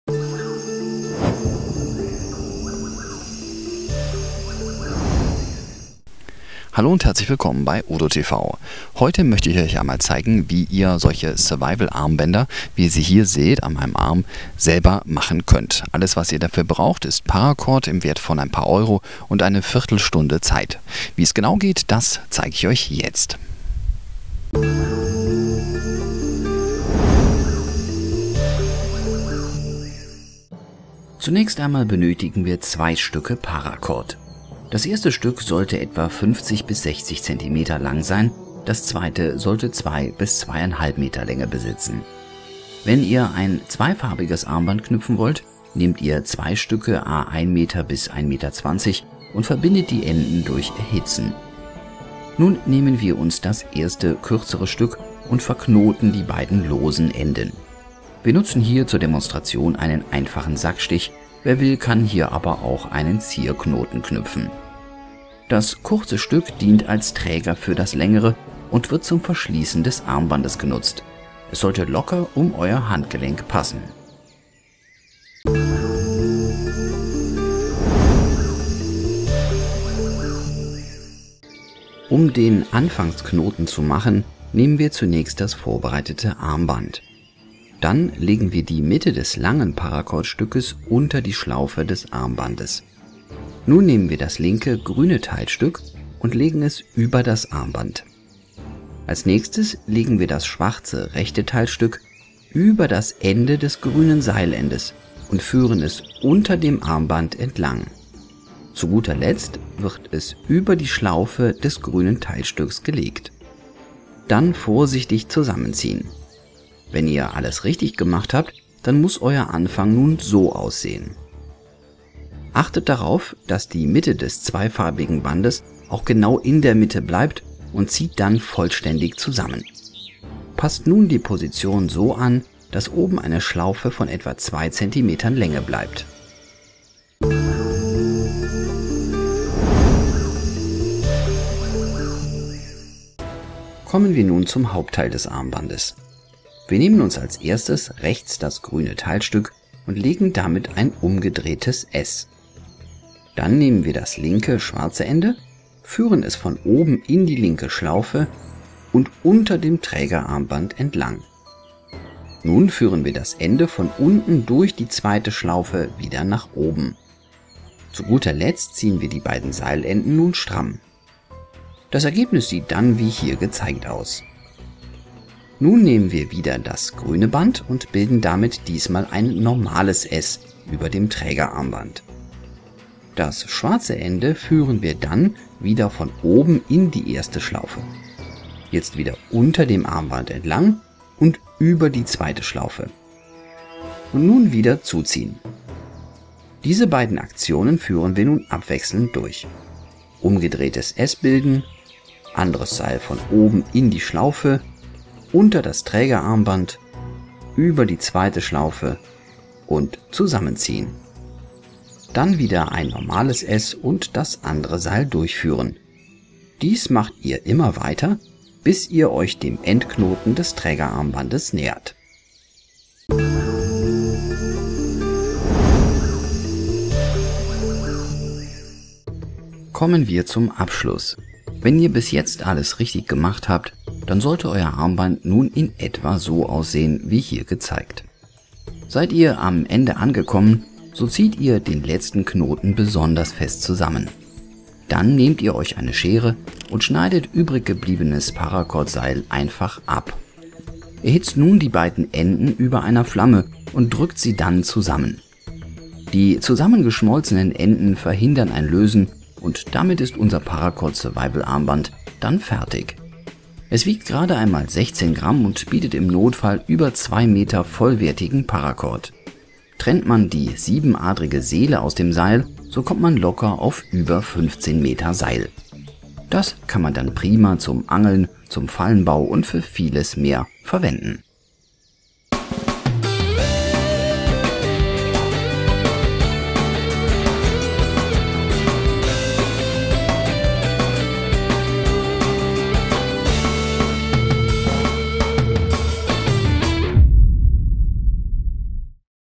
Paracord #04 - Survival Armband
In vielen Situationen kann es nützlich sein etwas Schnur dabei zu haben. Im Outdoor- und Survivalbereich bietet sich hier 550er Paracord Schnur besonders an. Sie ist robust genug um schwere Lasten zu halten und kann dank ihrer mehradrigen Seele leicht um den Faktor 8 verlängert werden. Um es auch im Alltag immer mit sich zu führen empfiehlt sich ein sogenanntes Paracord Survival Armband. Wir zeigen euch heute, wie es geknüpft wird.